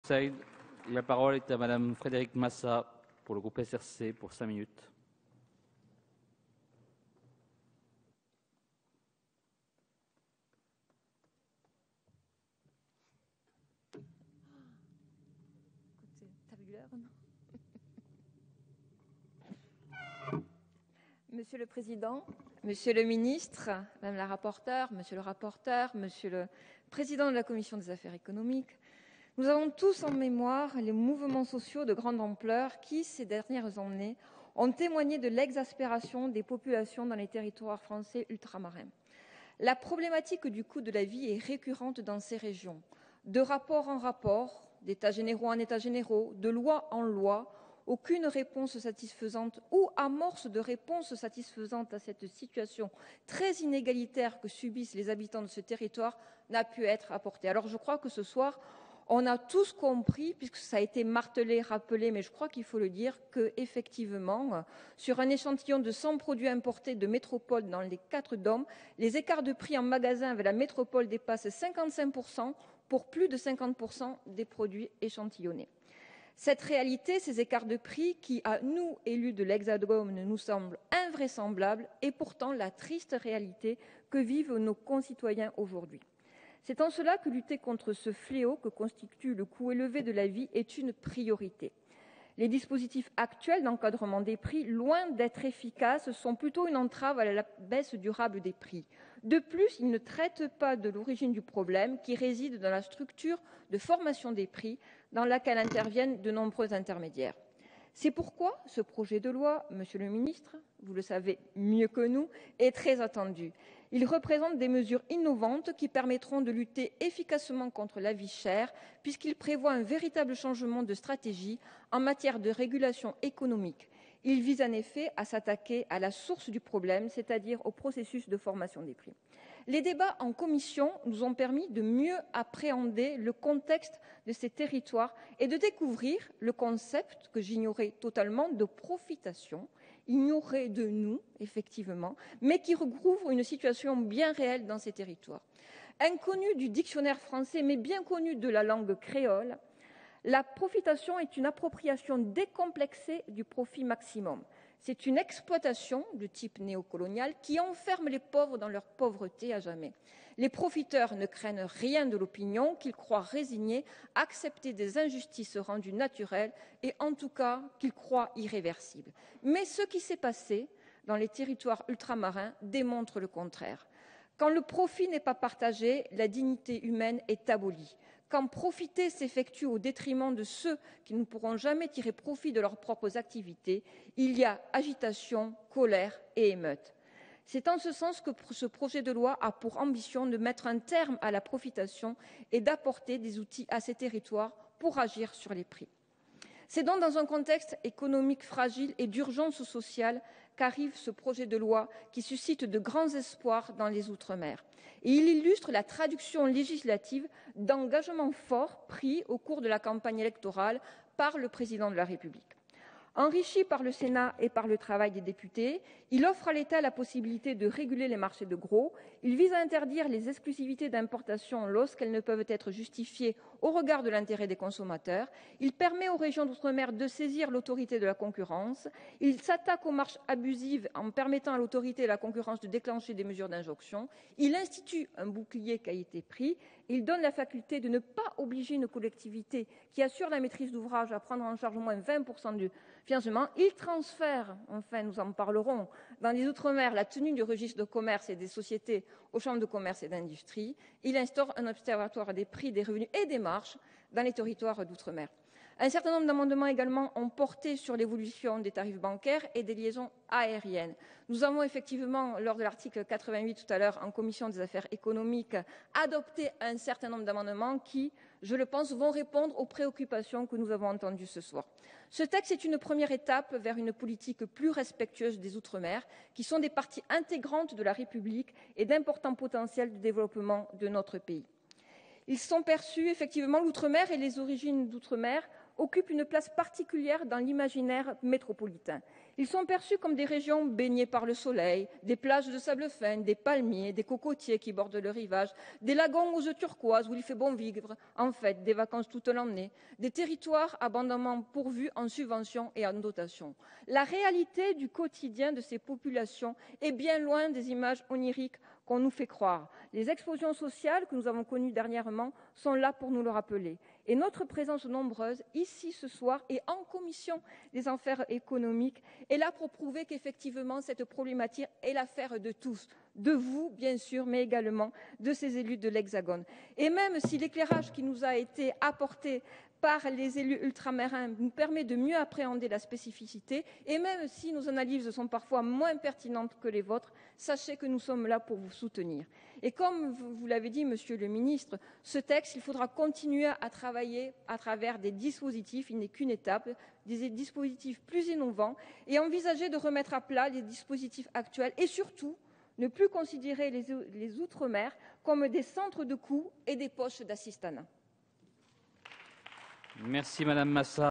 Projet de loi relatif à la régulation économique outre-mer
Mardi 9 octobre, l'Assemblée nationale a débuté l'examen du Projet de loi relatif à la régulation économique outre-mer et portant diverses dispositions relatives à l'outre-mer. Retrouvez l'intervention de Frédérique Massat dans la discussion générale.